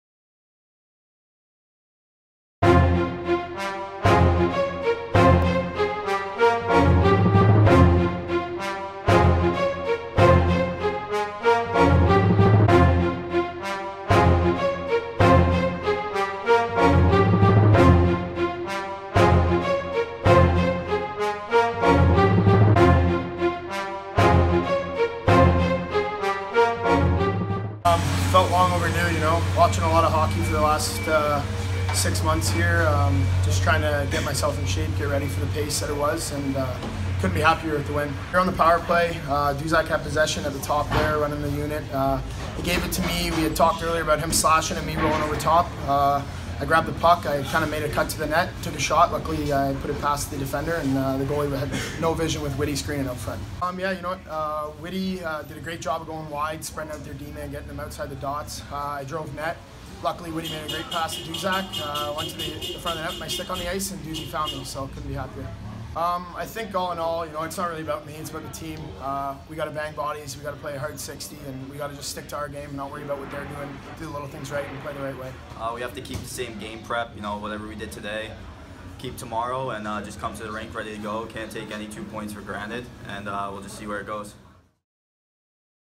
Lakers Win First of Two Games Against SHU
The Mercyhurst men's hockey team defeated Sacred Heart 5-3 with a pair of goals from sophomore Taylor Best. Interviews with Best and freshman Joey Duszak.